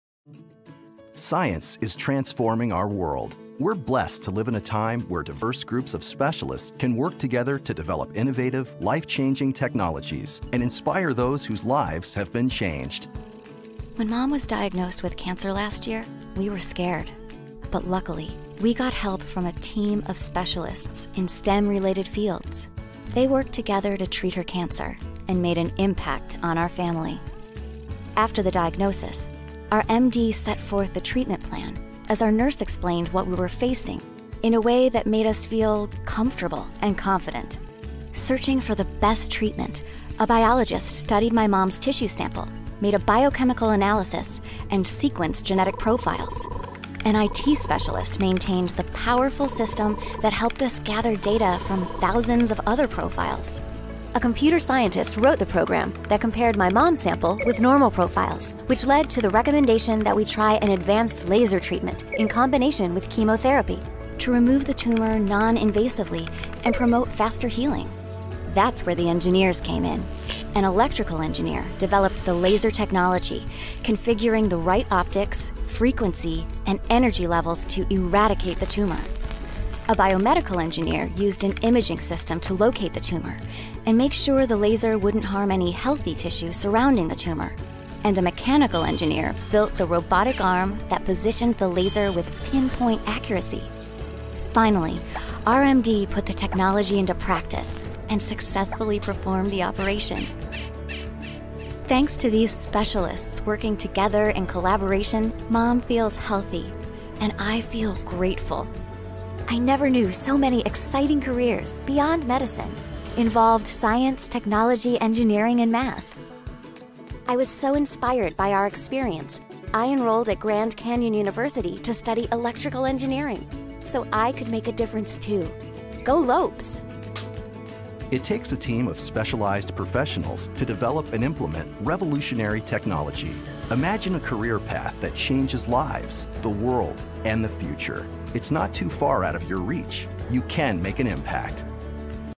Ngành STEM là gì?
- Đây là video đầu tiên trong chuỗi các video hướng nghiệp giúp các em dễ dàng hình dung hơn về công việc yêu thích và định hướng tương lai. Video này sẽ giới thiệu chung về ngành STEM và đưa ra ví dụ cách mà các ngành nghề kết hợp với nhau để tạo ra những thành quả khoa học công nghệ giúp ích cho cuộc sống.
- Nguồn video thuộc về Grand Canyon University.